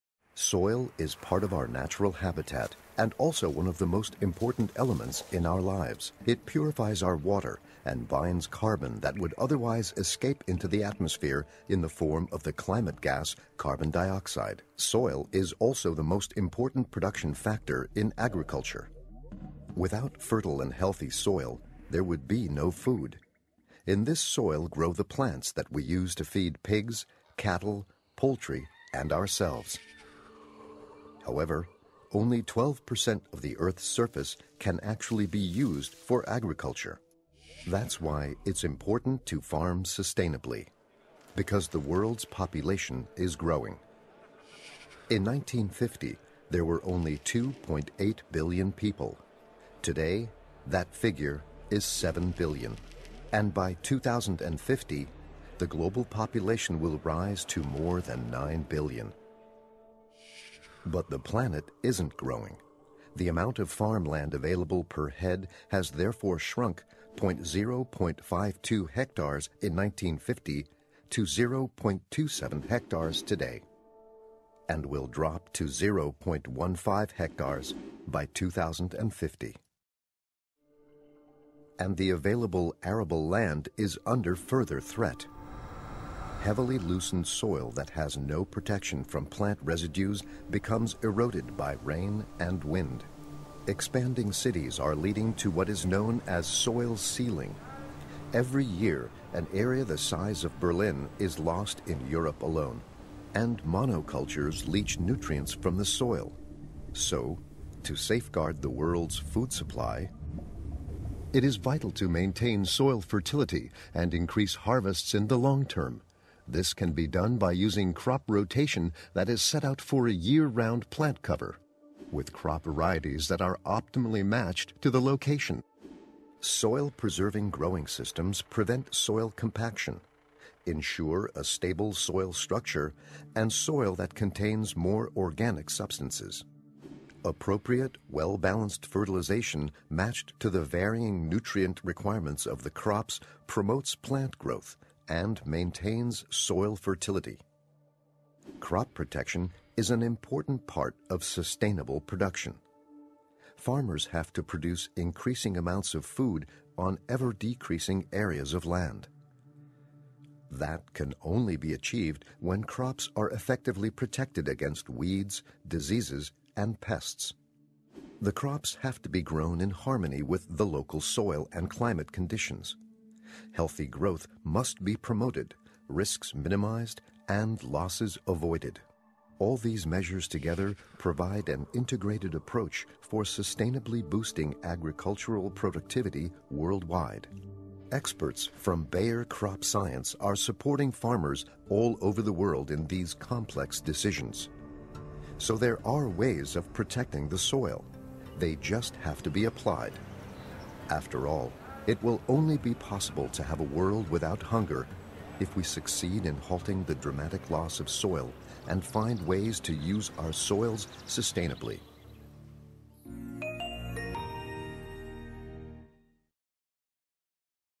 No Life Without Soil | Bayer
Without fertile and healthy soil, there would be no food.
And the significance of this vital resource is only expected to rise, as global challenges continue to grow.
 Will there be enough soil for future generations? There could be—provided that we succeed in managing our arable land efficiently. Watch to see how Bayer is actively working towards conserving our soil health.